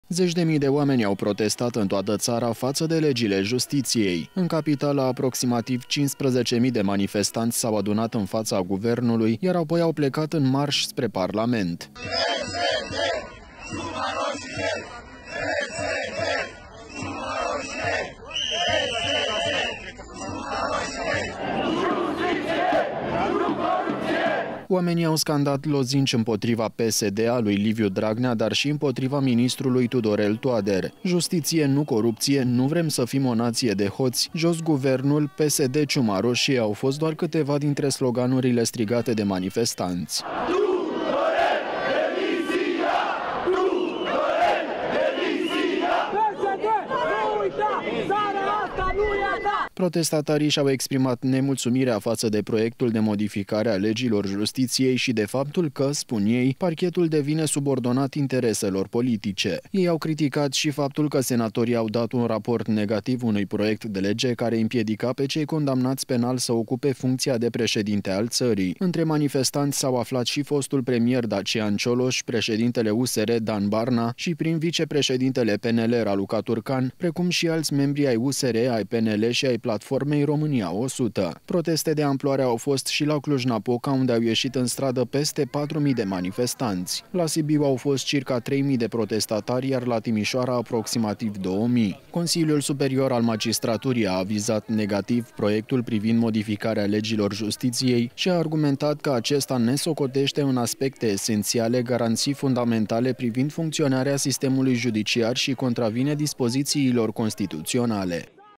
Proteste ample provocate de intenția de modificare a legilor Justiției
Zeci de mii de oameni au protestat în toată ţară de legile justiției! În Capitală, aproximativ 15.000 de manifestanţi s-au adunat în faţa Guvernului, iar apoi au plecat în marş spre Parlament. Oamenii au scandat lozinci împotriva PSD, a lui Liviu Dragnea, dar şi împotriva ministrului Tudorel Toader. ”Justiţie, nu corupţie”, ”Nu vrem să fim o naţie de hoţi”, ”Jos Guvernul”, ”PSD ciuma roşie”, au fost câteva dintre sloganurile strigate de manifestanţi. Protestatarii şi-au exprimat nemulțumirea faţă de proiectul de modificare a legilor justiției și de faptul că, spun ei, Parchetul devine subordonat intereselor politice. Ei au criticat și faptul că senatorii au dat un raport negativ unui proiect de lege care-i împiedica pe cei condamnaţi penal să ocupe funcţia de preşedinte al ţării. Între manifestanţi s-au aflat fostul premier Dacian Cioloş, preşedintele USR, Dan Barna, şi prim-vicepreşedintele PNL Raluca Turcan, precum şi alţi membri ai USR, ai PNL şi ai Platformei România 100. Proteste de amploare au fost şi la Cluj-Napoca, unde au ieşit în stradă peste 4.000 de manifestanţi. La Sibiu au fost circa 3.000 de protestatari, iar la Timişoara, aproximativ 2.000. Consiliul Superior al Magistraturii a avizat negativ proiectul privind modificarea legilor Justiţiei și a argumentat că acesta „nesocoteşte” în aspecte esenţiale garanţii fundamentale privind funcţionarea sistemului judiciar şi contravine dispoziţiilor constituţionale.